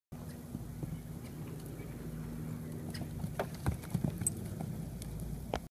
Day 3 - Jumping Practice with Miss Carol
Excited and proud of Stormy for all of his progress during Camp Carol. Thanks to everyone who loves and supports me and my crazy horse.